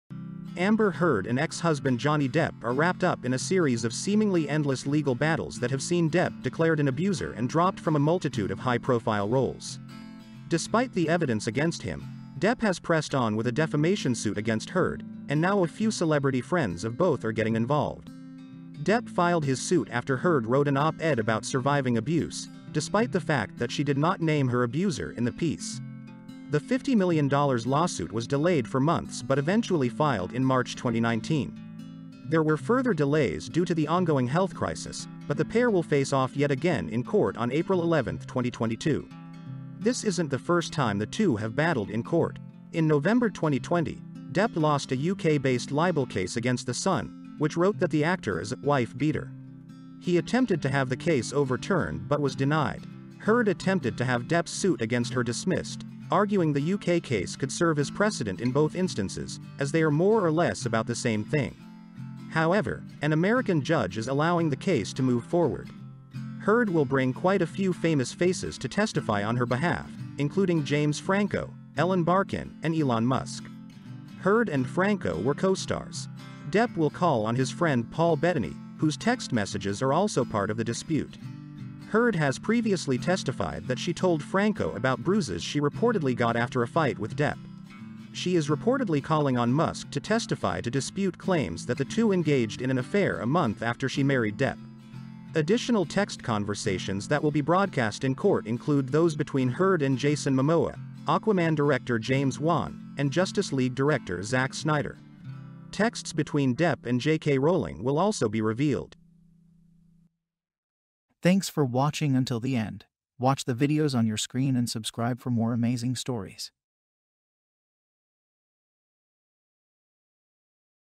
Elon Musk Will Testify On Behalf Of Amber Heard In Abusive Ex Husband's Suit Against Her
Subscriber For daily Videos About Different Topics with unbelievable facts that'll make you smarter and more well informed.